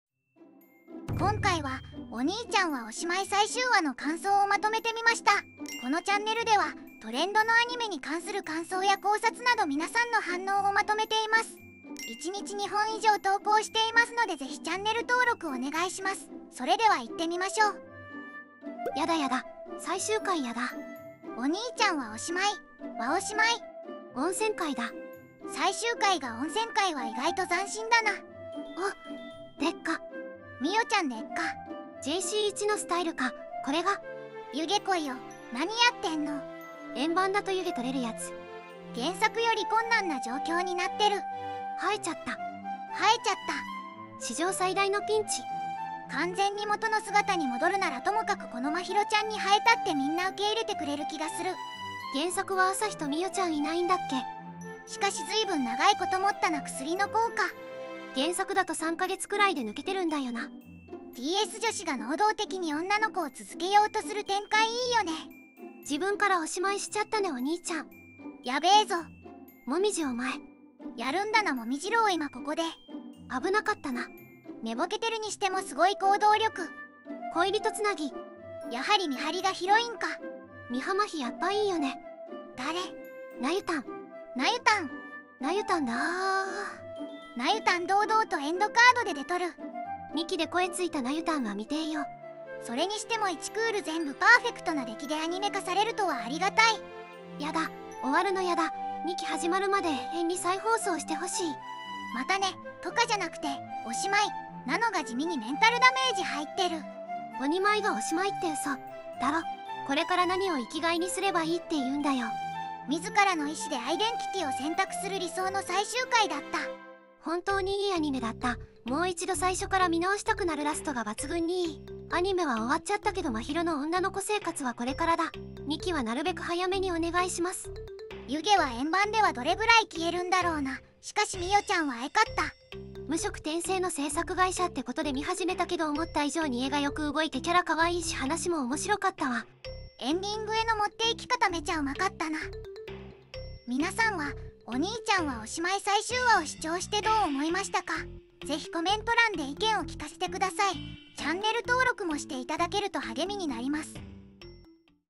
【最終回】エッ〇な温泉回に神エンディング... おにまい第12話感想まとめ!!【2ch/反応】
流行りのアニメを中心に反応集の動画をアップしていきます

©ねことうふ・一迅社／「おにまい」製作委員会